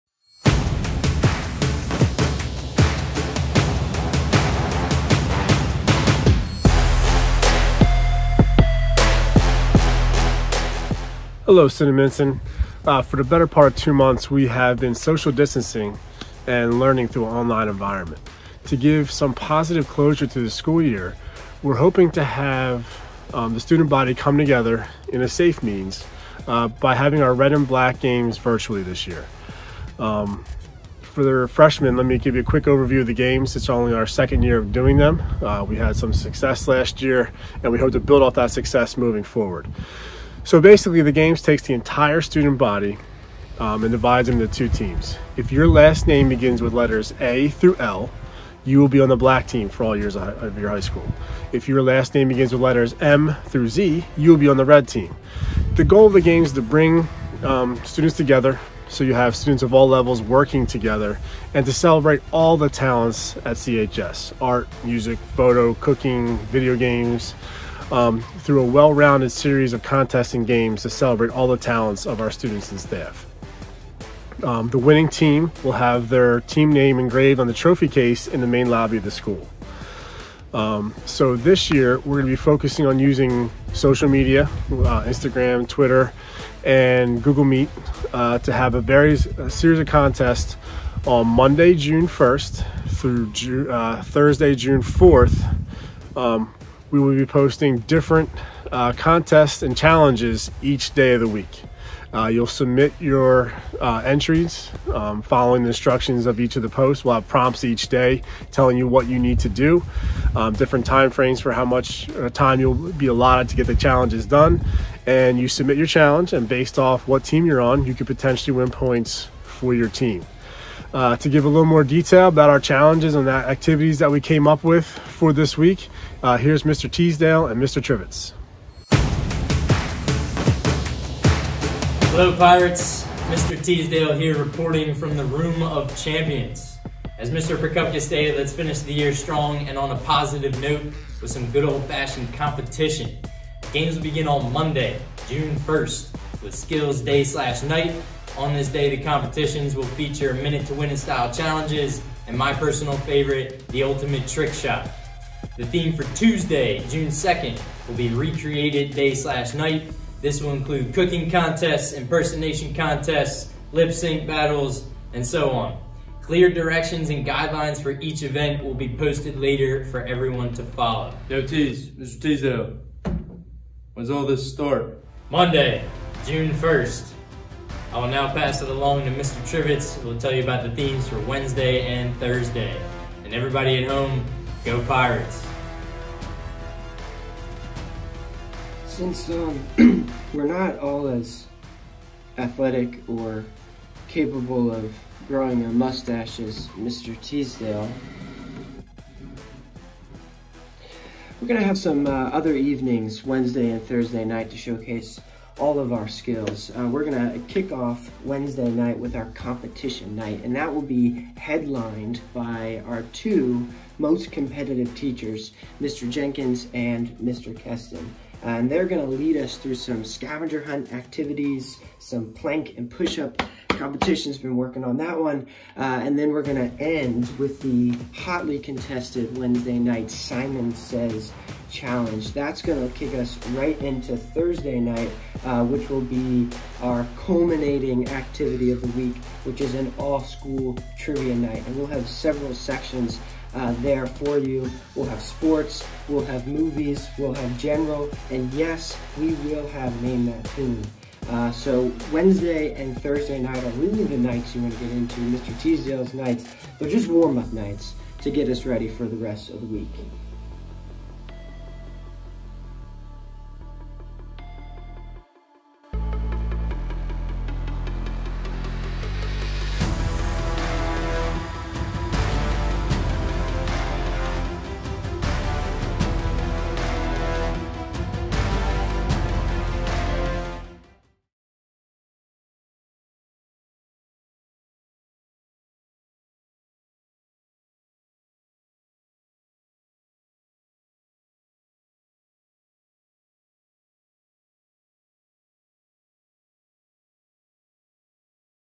CHS 2020 Red & Black Games Promo
Information regarding this year's Red & Black Games, happening June 1st-4th